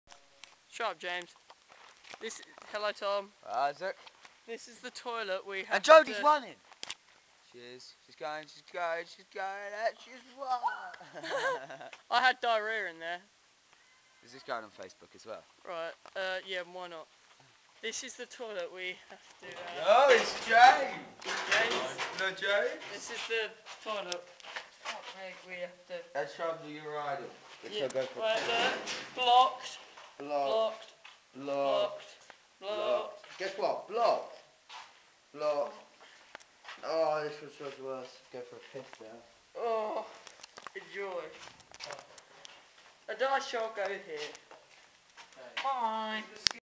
Namibia, Africa.
Me and 14 other students travelled to Namibia to help refubish a school. It was called !Oegab. Good times  2009.